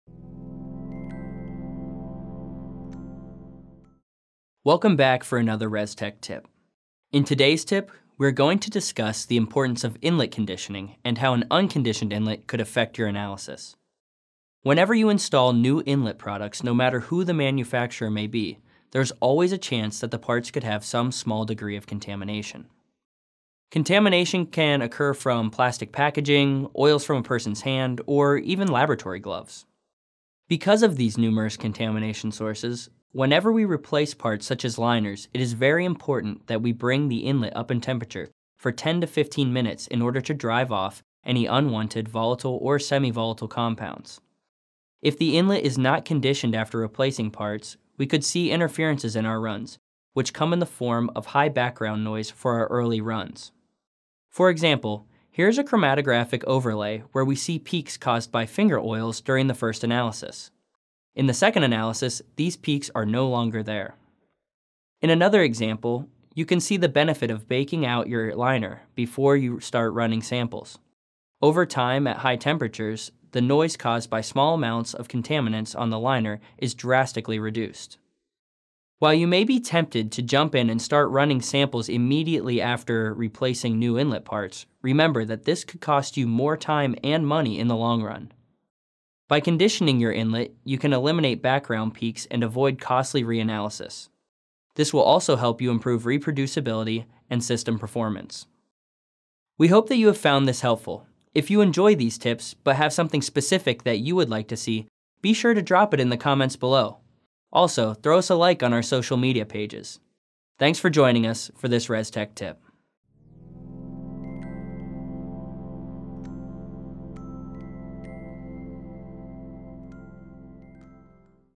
Conditioning GC Inlet Parts
Conditioning is a critical step in gas chromatography that helps to remove contaminants. Many people have been conditioning their columns for years but may not realize it is also important to condition the inlet after installing new consumables like liners, inlet seals, and O-rings. Replacement parts may have contamination from their manufacture, packaging, or even laboratory gloves, and a short inlet conditioning is a very effective way to drive off this contamination and reduce any impact that it might have on your analysis.

Additional Resources